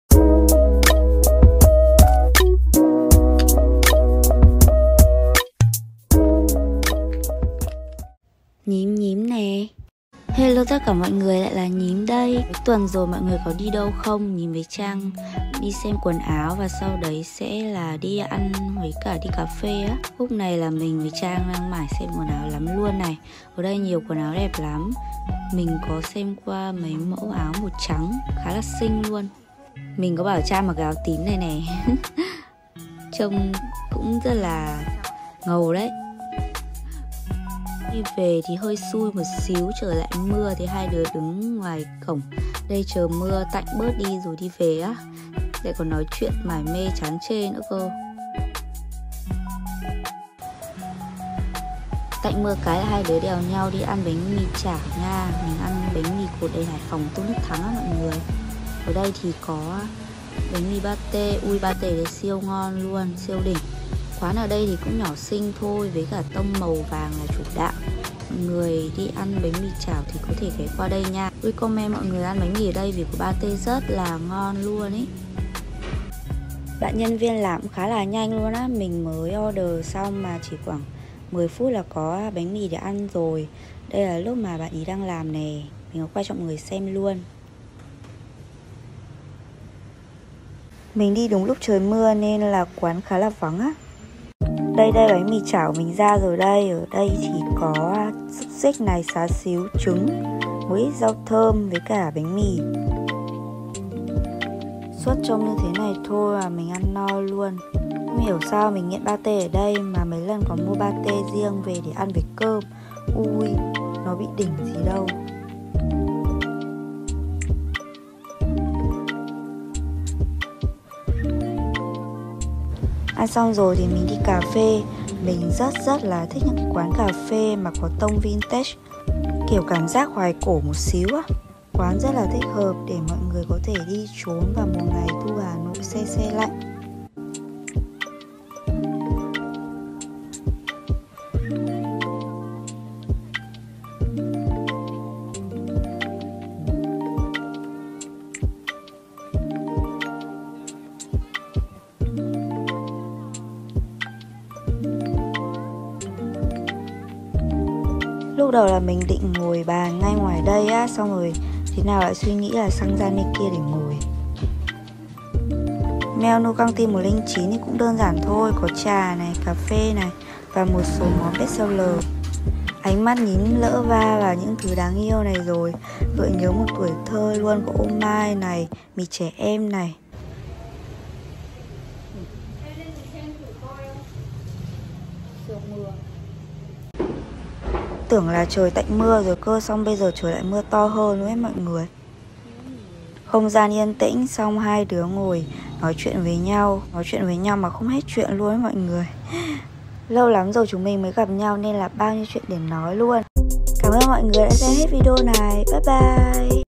Một quán cafe tone vintage, thích hợp chill ngày cuối tuần một ngày Hà Nội vào thu?
Hi, Nhím đây!
FACEBOOK: Vũ Thị Thanh Hiền